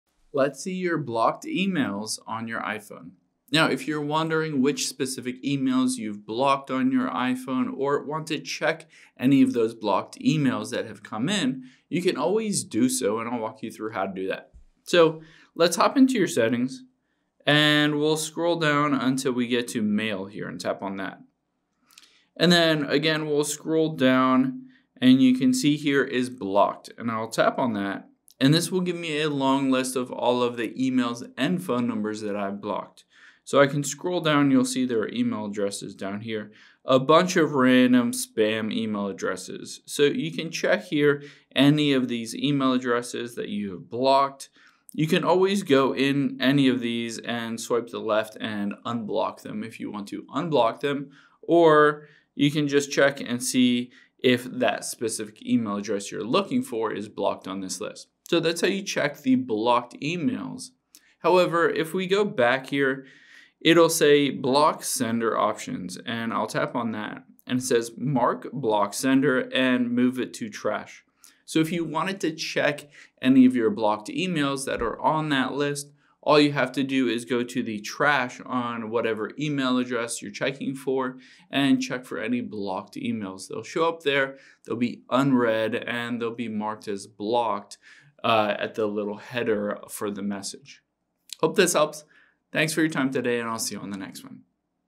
How To See Blocked Emails On iPhone
Let's find all the email addresses you blocked on your iPhone in case you want to unblock them or add a new email.